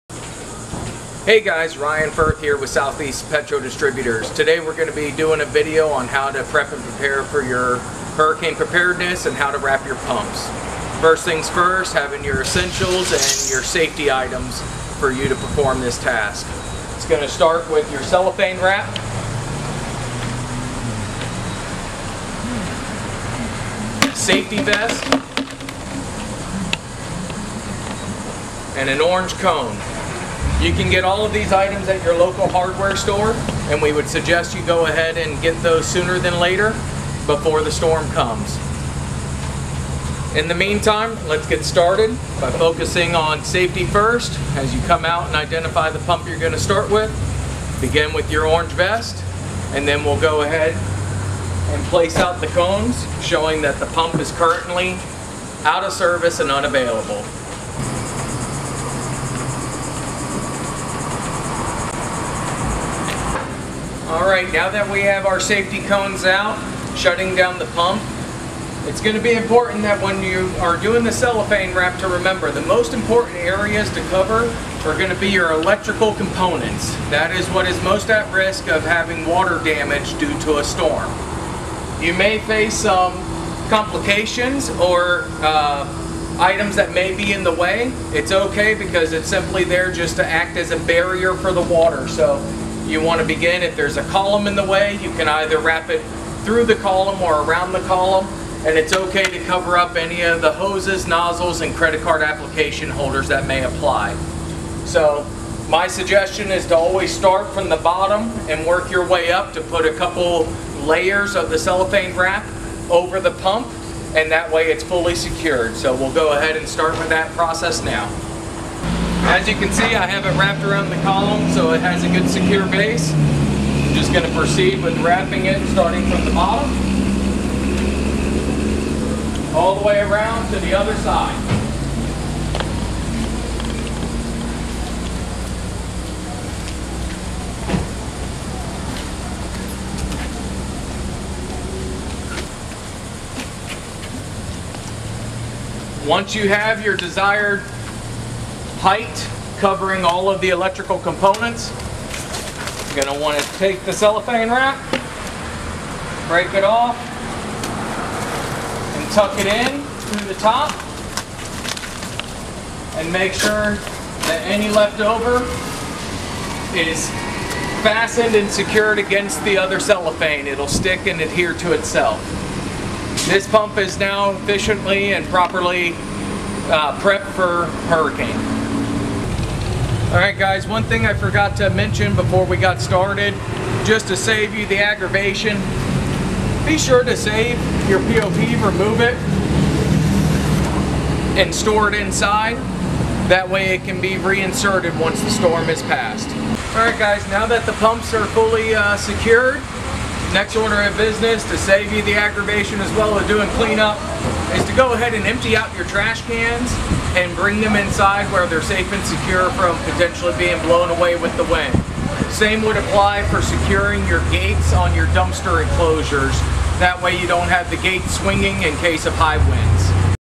How to Wrap Gas Pumps in Preparation for a Hurricane | Southeast Petro
In the event that a storm event is severe enough to warrant shutting down your gas station, there are certain preparations that need to be made in order to secure your gas pumps to prevent damage. Wrapping your pumps with saran wrap will secure the hoses and nozzles from flying off, potentially banging them into the pump during high winds, and it will also protect from water intrusion of the electrical components.

Make sure to follow the safety protocols when out securing your pumps by wearing a safety vest and using cones. Wrap the pumps from the bottom up to ensure that the water will run off the saran wrap and not seep in between the seams.